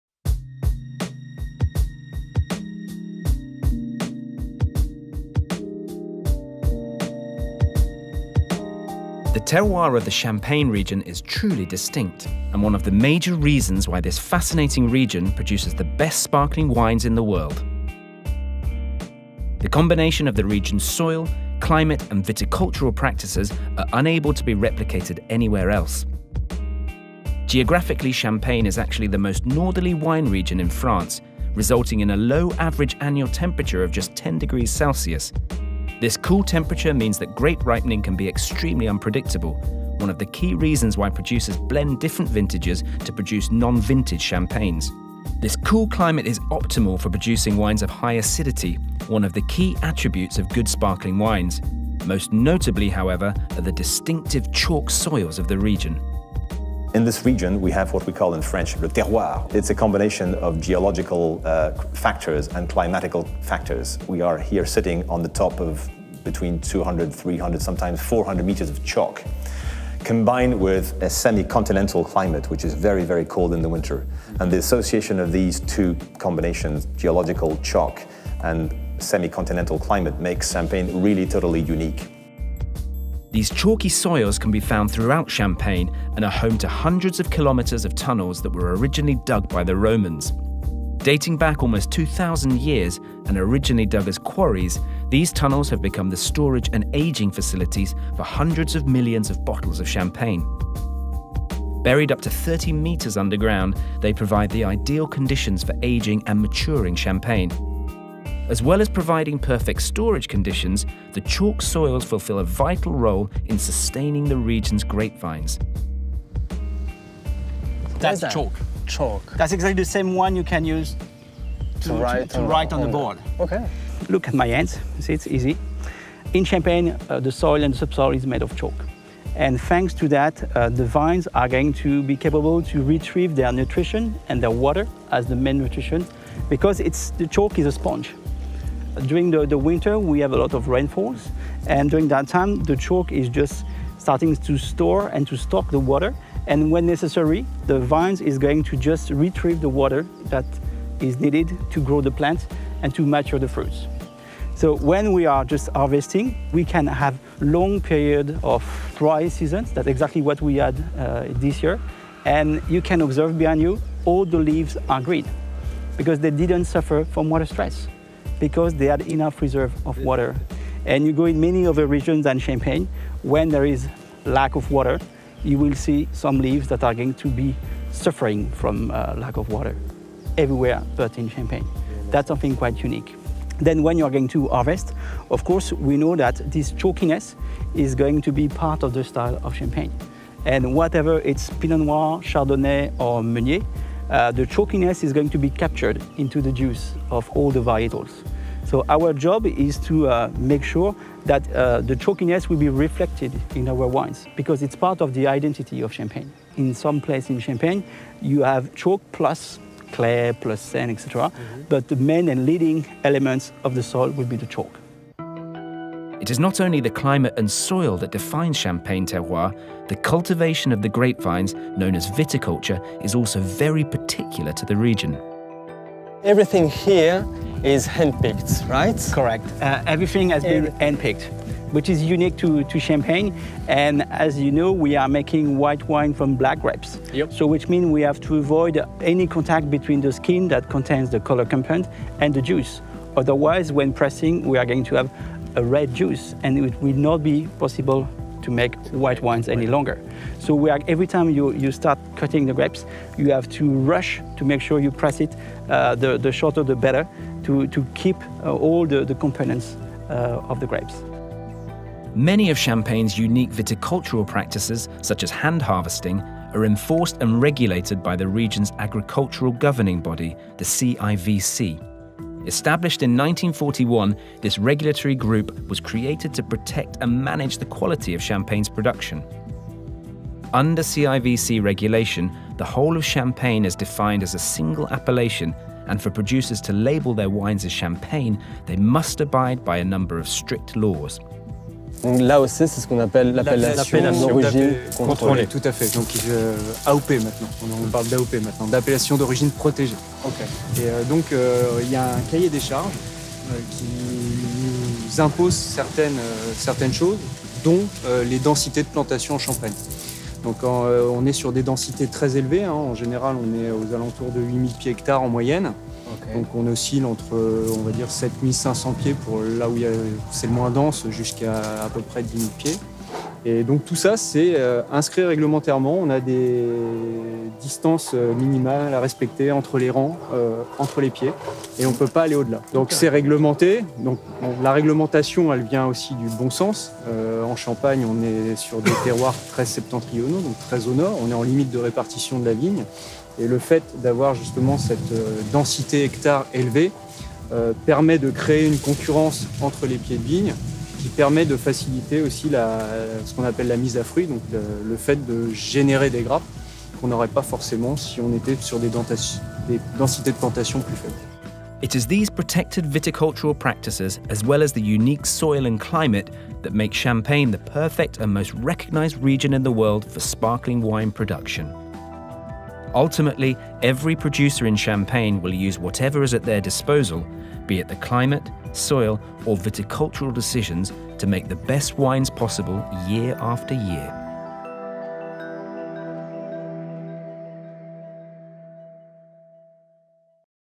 The Unique Terroir Of The Champagne Wine Region
Did you know that only the region of Champagne, in France, has the only combination of soil, climate and viticultural practices that let it produce one of the best sparkling wines in the world? Learn more about the indispensable factors required to make what is called the champagne.